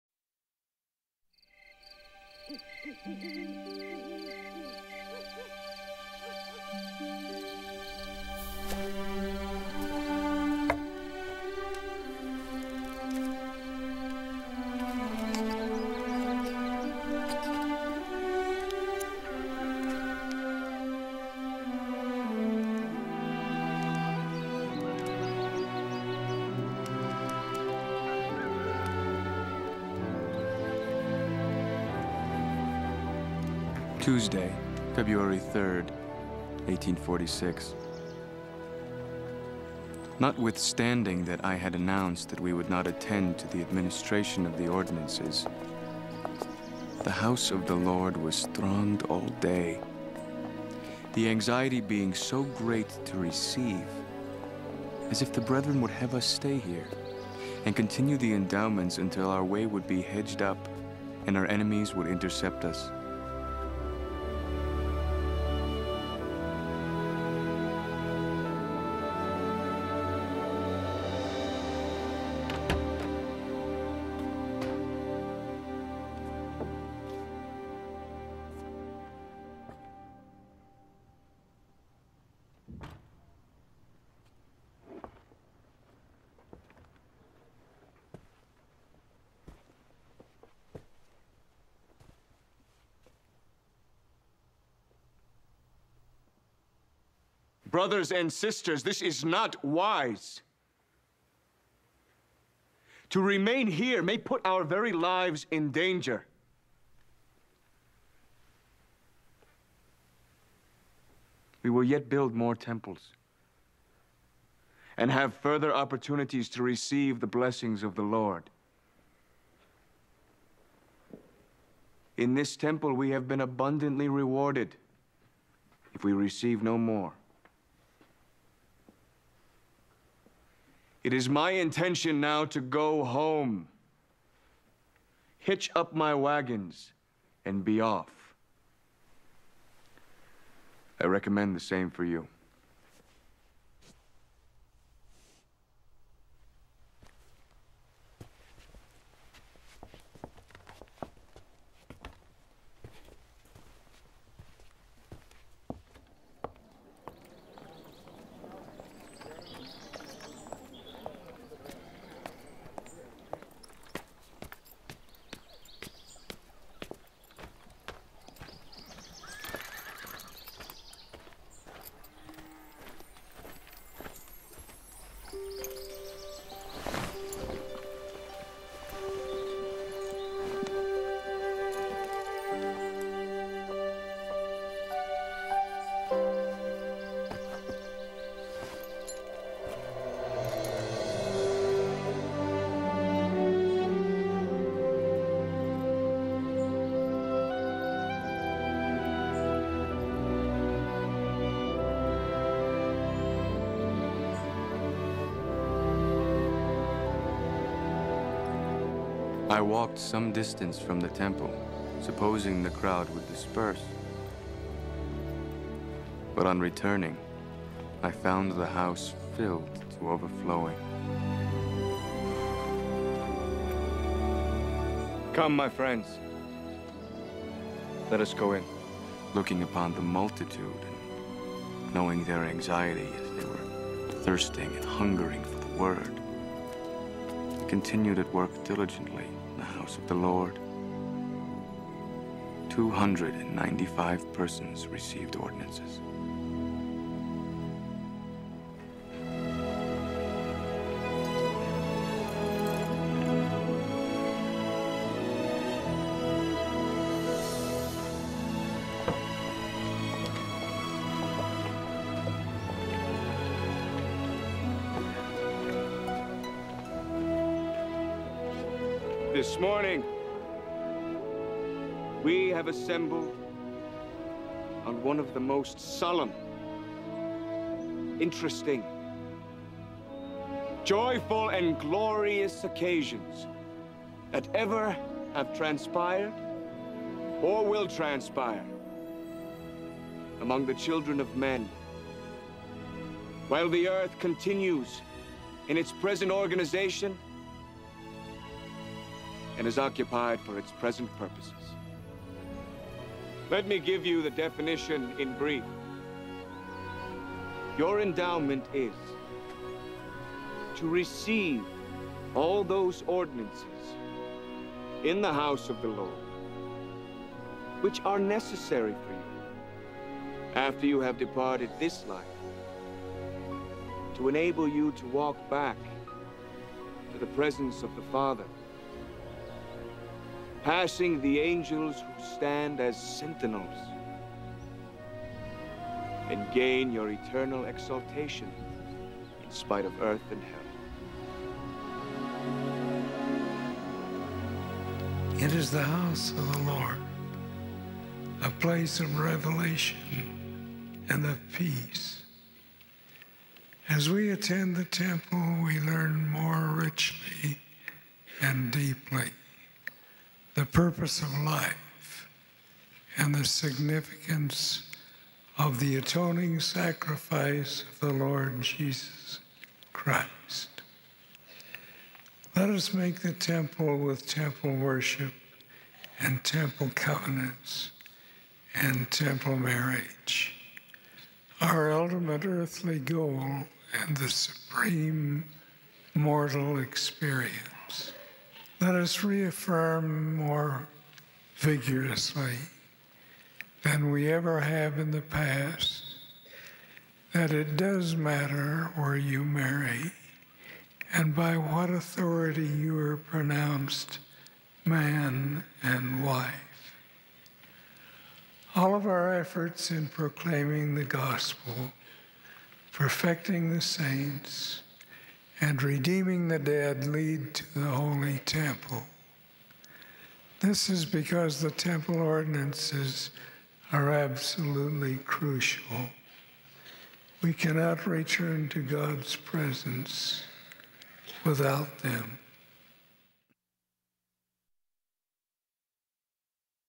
Endowed With Power
President Brigham Young speaks of the saints receiving Temple ordinances before leaving Nauvoo. President Howard w Hunter and others speak of the blessing of participating in Temple ordinances.